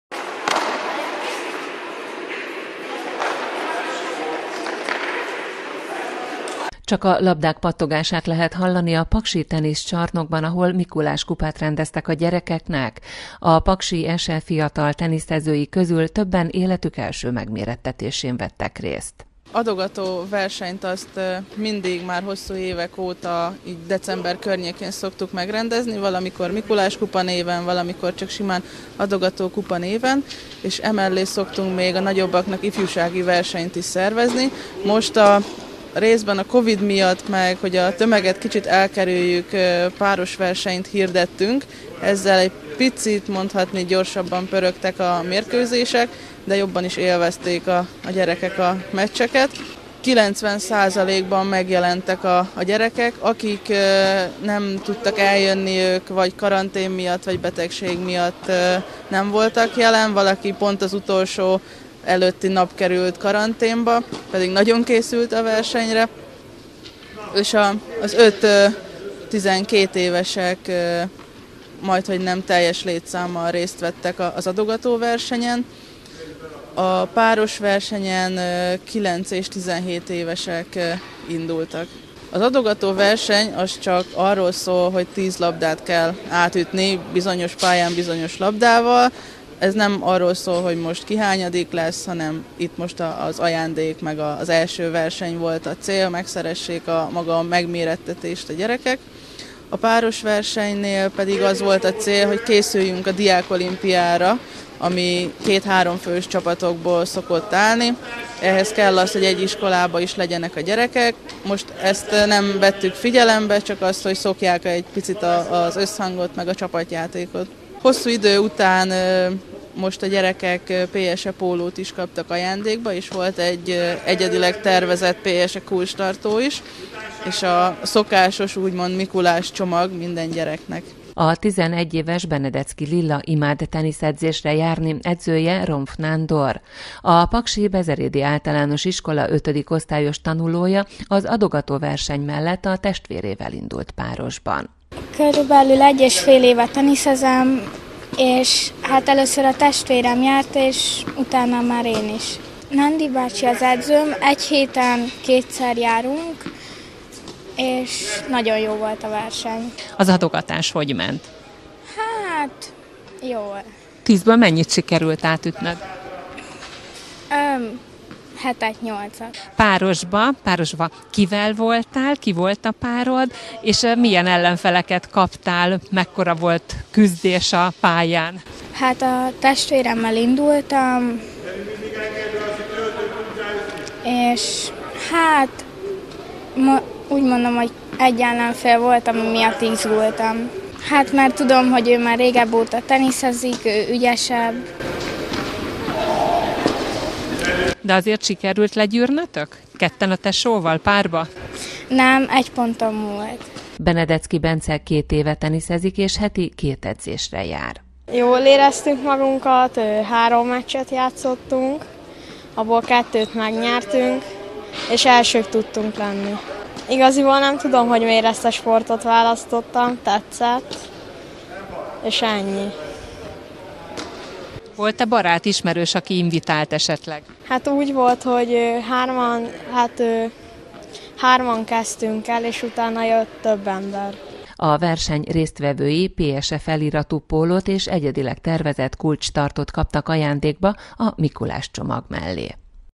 Mikulás kupát szervezett a Paksi SE tenisz szakosztálya
Adogató és ifjúsági páros teniszversenyt rendezett a Paksi SE tenisz szakosztálya Mikulás Kupa néven. A megmérettetésen több tucatnyi fiatal vett részt. Az 5-12 éves korosztály adogató versenyen mérettette meg magát, míg a 9-17 évesek páros versenyen vettek részt.